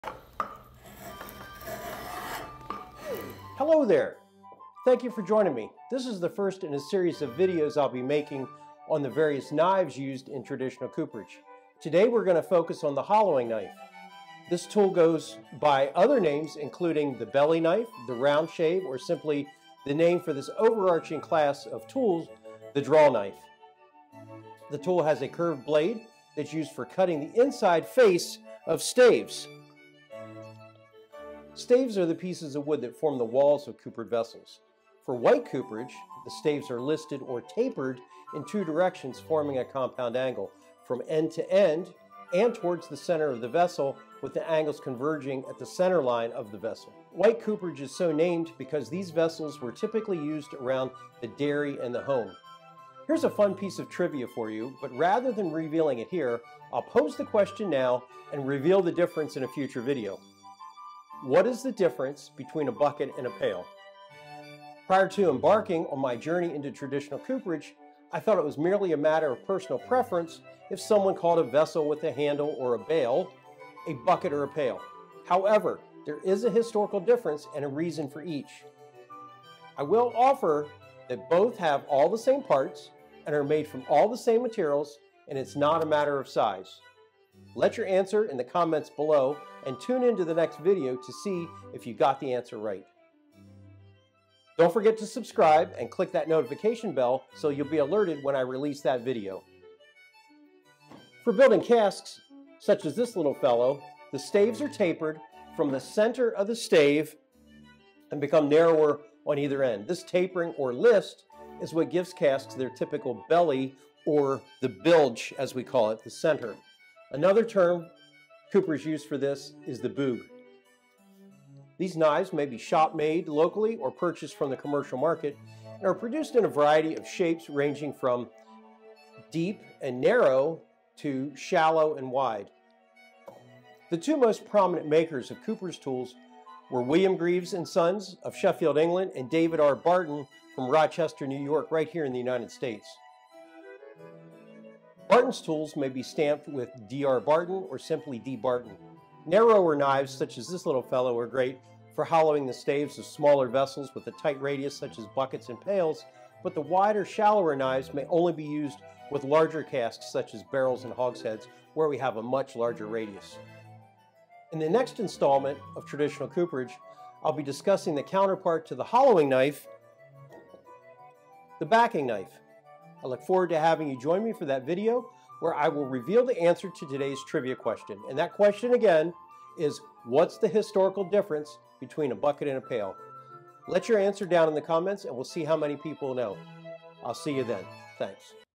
The Cooper’s Hollowing Knife: An Elegant Tool From a More Civilized Age
The Hollowing Knife is used for cutting the concave inner face of staves used in the construction of coopered vessels, ranging from meter buckets to whiskey casks, here at the historic Dilll's Tavern in Dillsburg, PA.

00:00 Introduction
00:15 Other names
00:24 Description
00:31 Staves
00:35 White Cooperage staves
01:00 Today's trivia question
01:39 Cask staves
02:00 Historical sources for Hollowing Knives
02:43 Coming up next, The Backing Knife